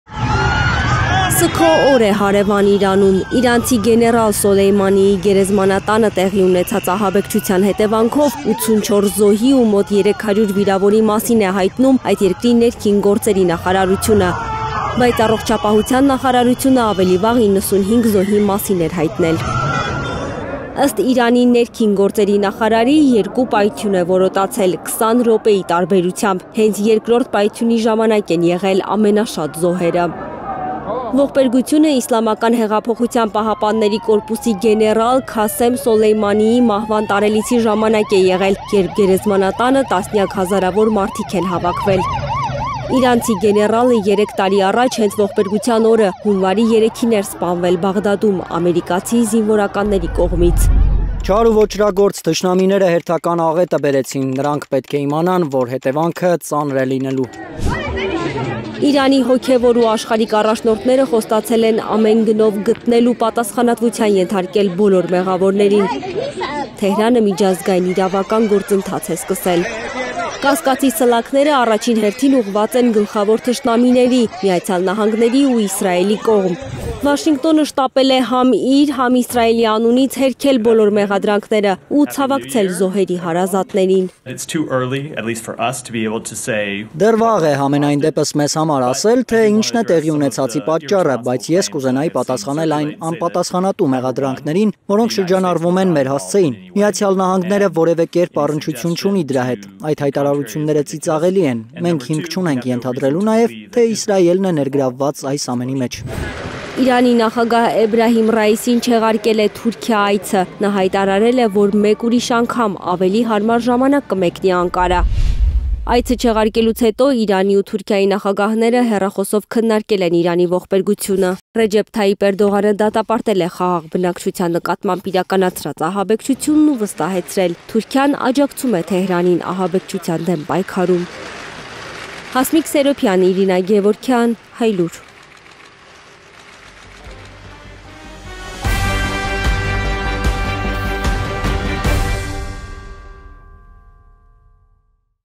Իրանը փնտրում է ոճրագործներին. ԱՄՆ-ը հերքում է, թե կապ ունի ահաբեկչության հետ
Մեր հարևան Իրանում սգո օր է: Իրանի Քերման քաղաքի գերեզմանատանը տեղի ունեցած պայթյունի հետևանքով պաշտոնապես 95 զոհի ինքնություն հաստատվել է, խոսվում է ավելի քան հարյուր զոհի և տասնյակ վիրավորների մասին: Իրանը միջազգային իրավական գործընթաց է սկսել, երկրի նախագահը խոստացել է, մեղավորներին գտնել և պատժել: Միացյալ Նահանգները պնդում է՝ ինքը կապ չունի և չի հավատում, թե Իսրայելի ձեռքի գործը կարող է լինել ահաբեկչությունը: